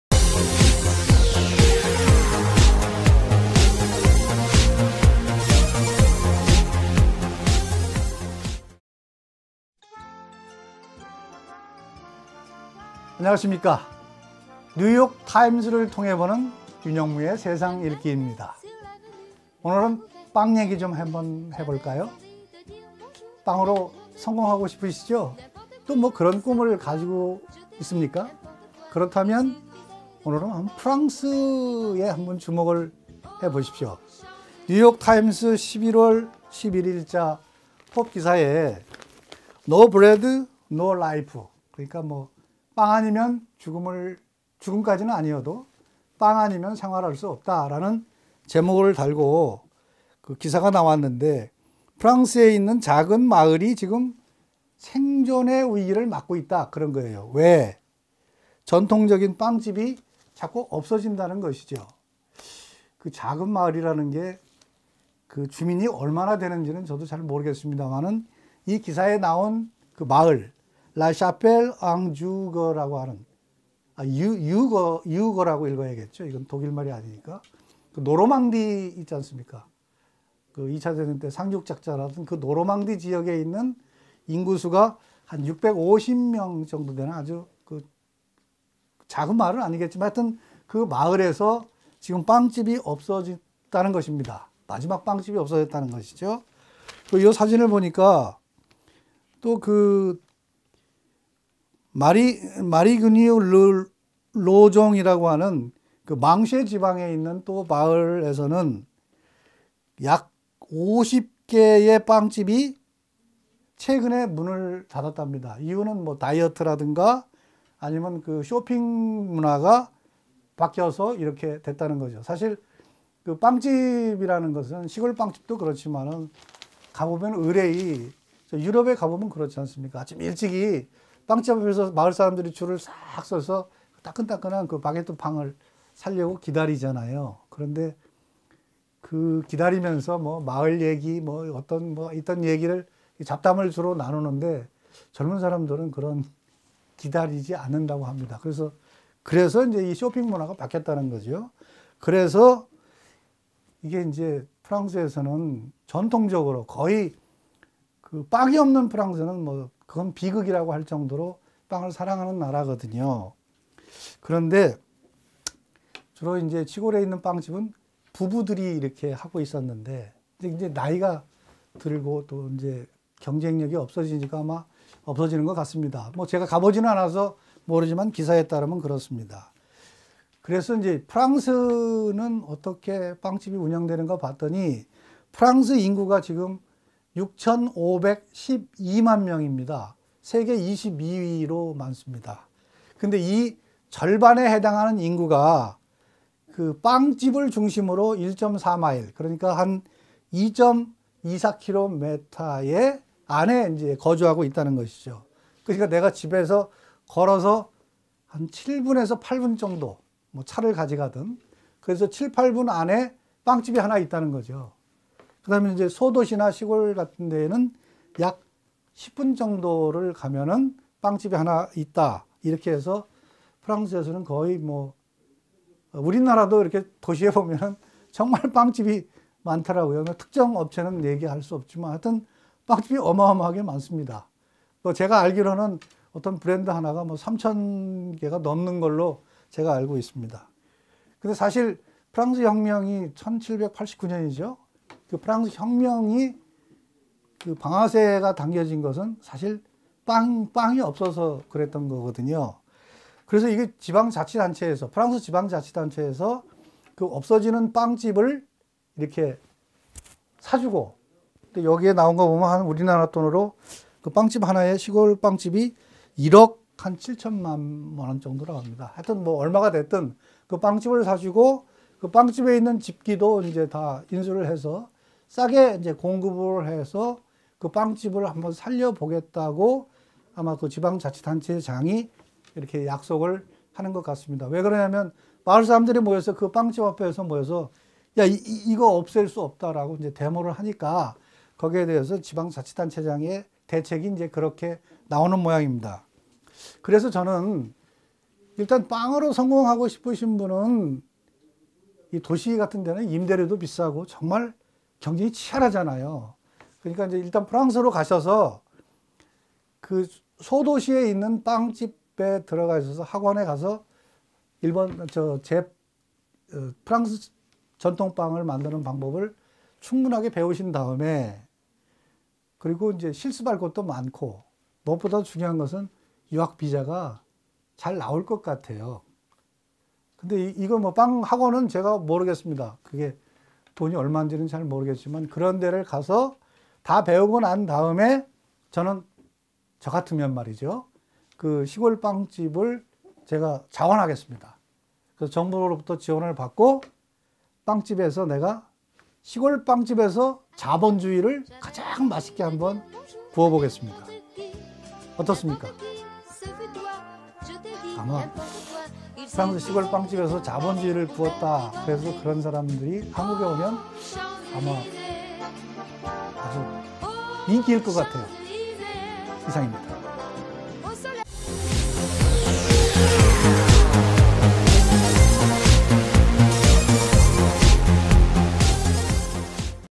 프랑스에서 자본주의를 구워보자 프랑스빵집이야기 윤영무 뉴욕타임스로보는세상읽기 국민의소리 한국신문방송인클럽
프랑스에서 자본주의를 구워보자! 시골 빵집이 사라지고 있는 프랑스 이야기